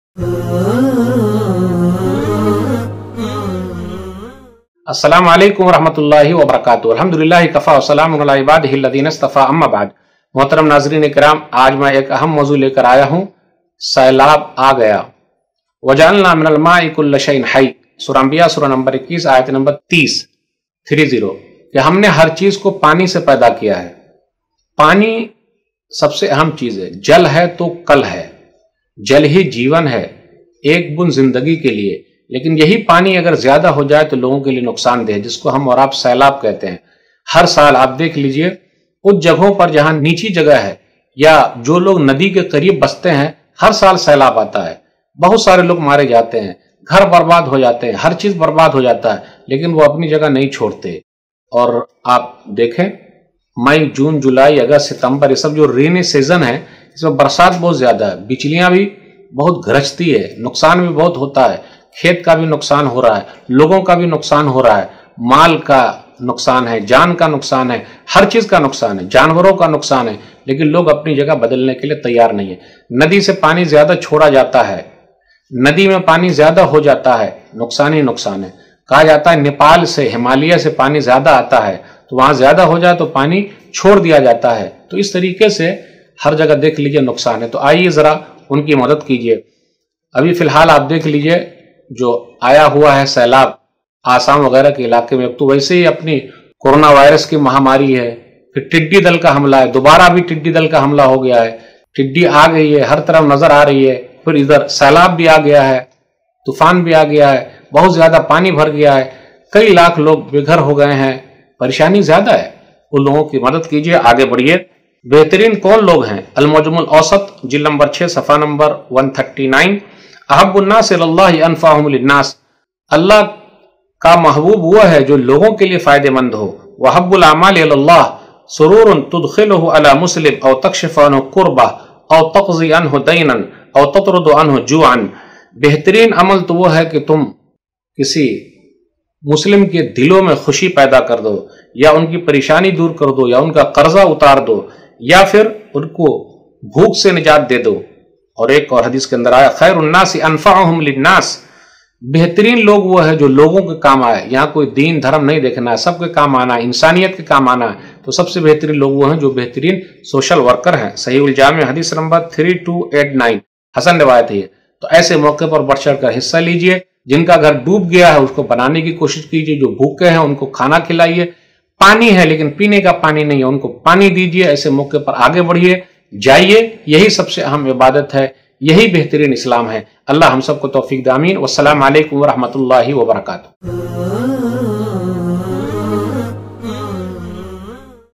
Flood in Assam | सैलाब की तबाही और हमारी जिम्मेदारी | Appeal By Shaikh Sanaullah Madani
Assalamu Alaikum Wa Rahmatullahi Wa Barkatuhu...

Aap Apne Sawaal Comments Mein Likh Sakte Hain, Unme Se Selected Sawaalon Ke Jawab Shaikh Sanaullah Madani Ke Zariye Diye Jaenge,
Insha ALLAH